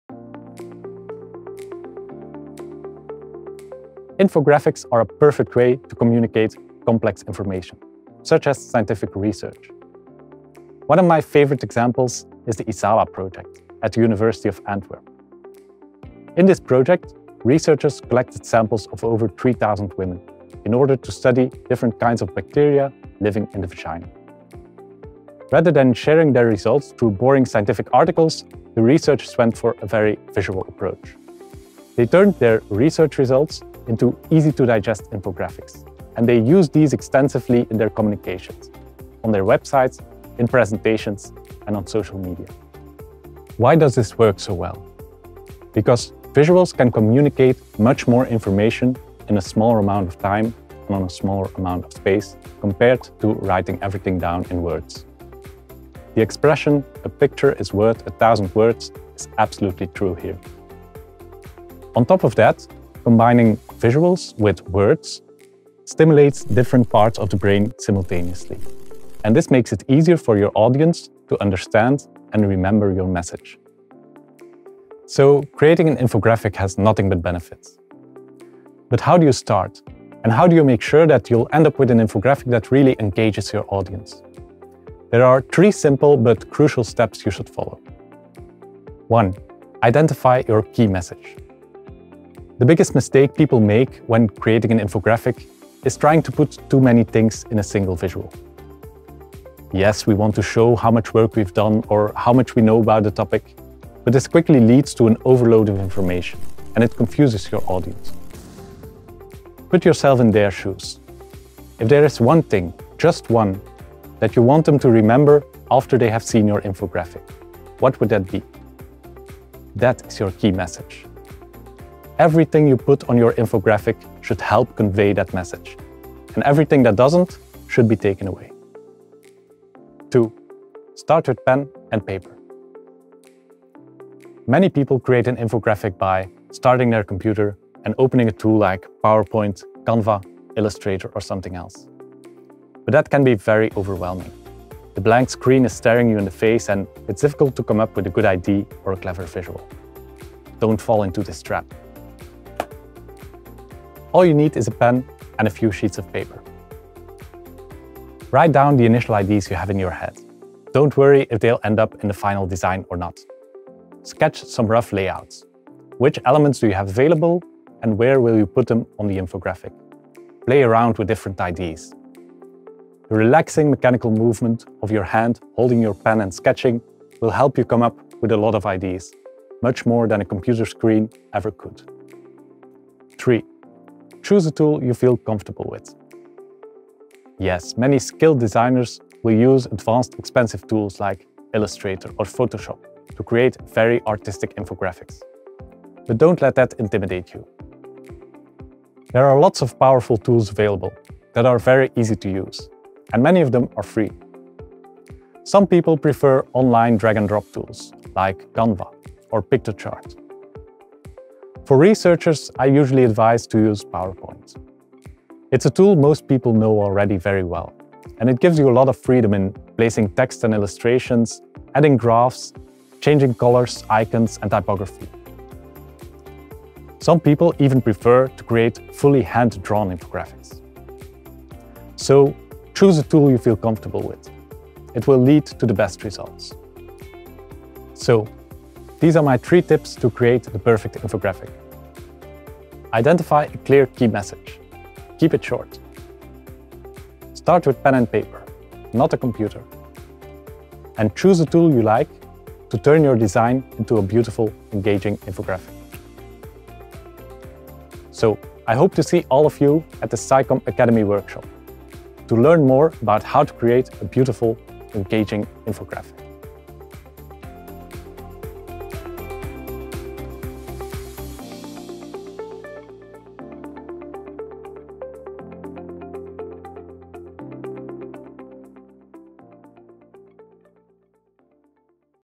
Three crucial steps to create your first infographic
Infographics are a perfect way to communicate complex information, such as scientific research. But how to start? Information designer Koen Van den Eeckhout gives you three crucial tips to get started.

Want to know more?